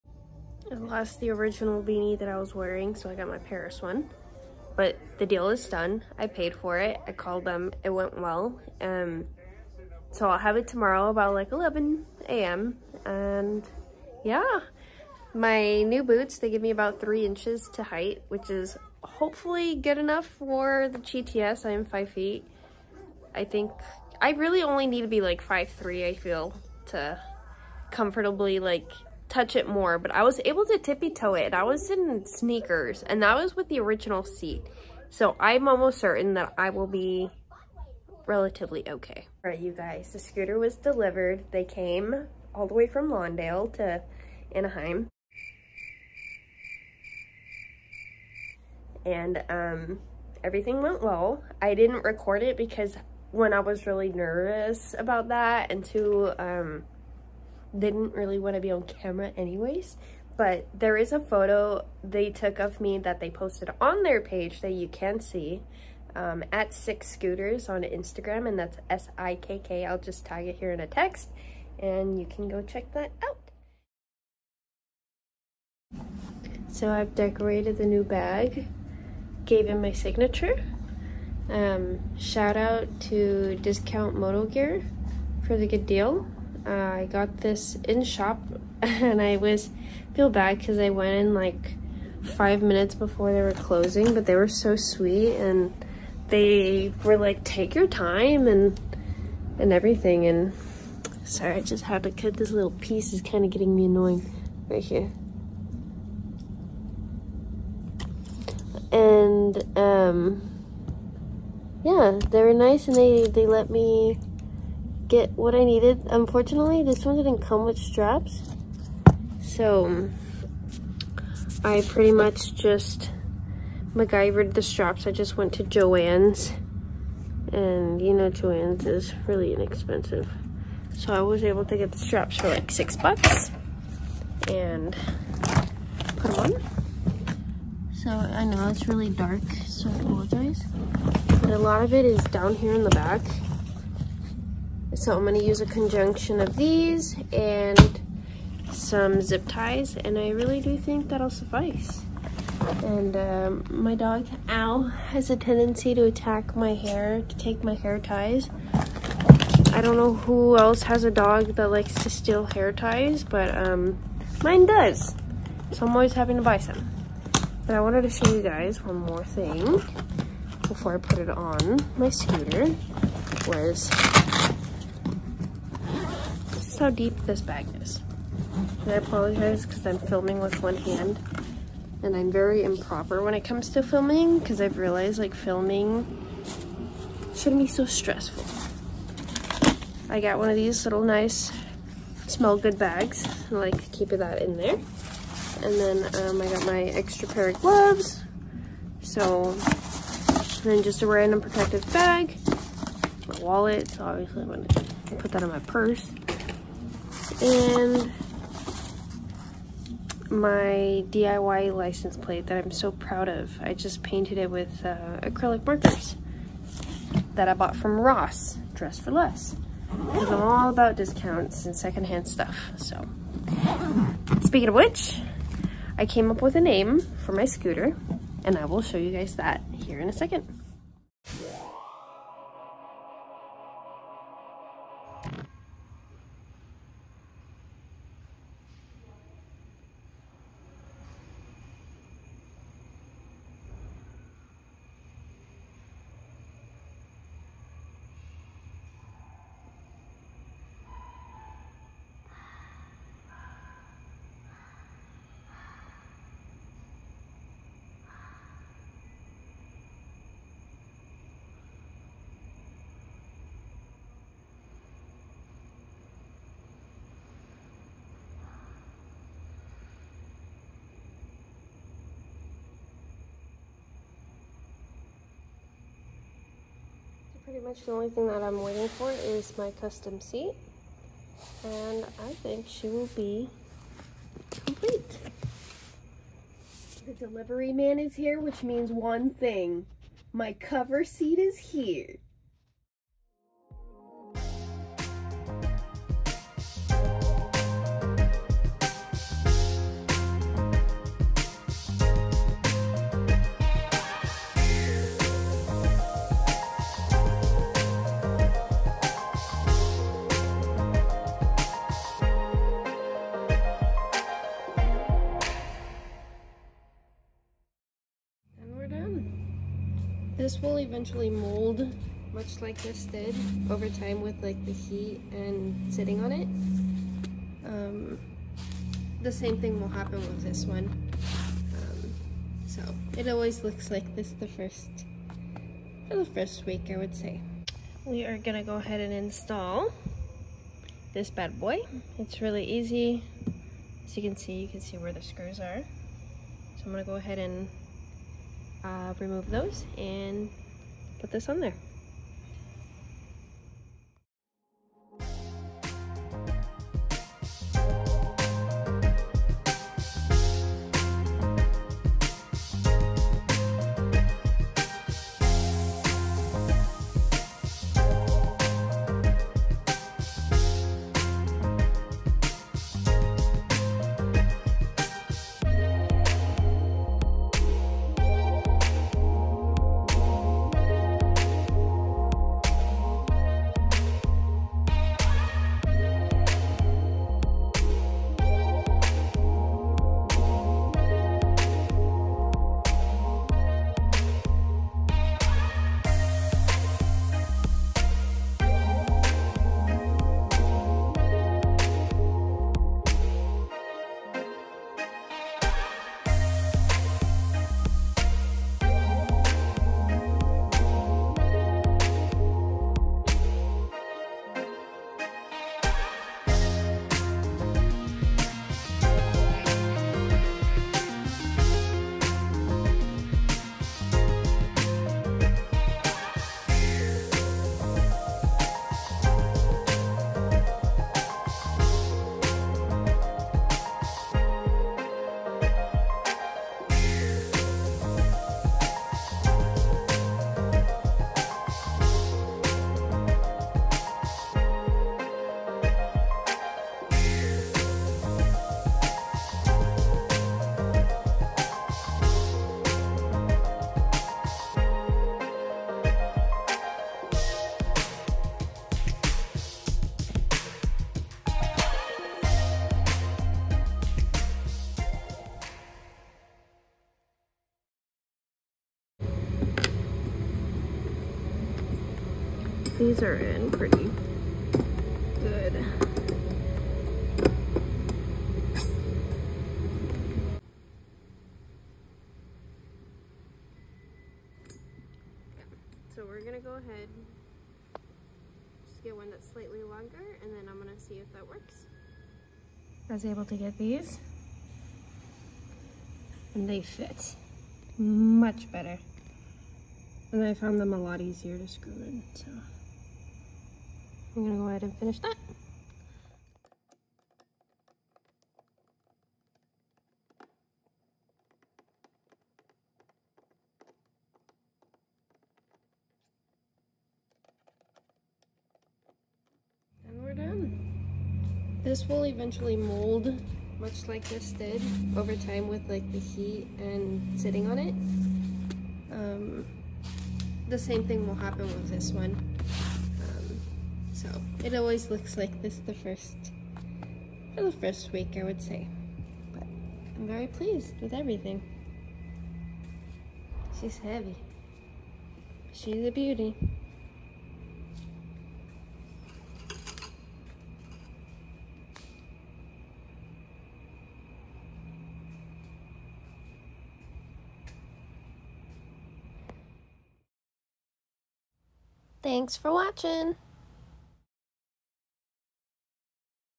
Vespa GTS 250ie | Purchase, Projects and Parts! | Ol' Reliable
Introducing Ol' Reliable, my new used scooter.
SKIP TO 00:04:00 to ignore the yapping

00:00:00-00:00:30 - Intro
00:00:30-00:00:58 - Delivery Discussion
00:00:58-00:04:00 - Projects, DIY, License Plate
00:04:00-00:04:45 - Ol' Reliable showcase
00:04:48-00:05:38 - Cheeky Seat Cover install
00:05:38-00:08:38 - Installation of Center Rack
00:08:38-00:09:27 - Close out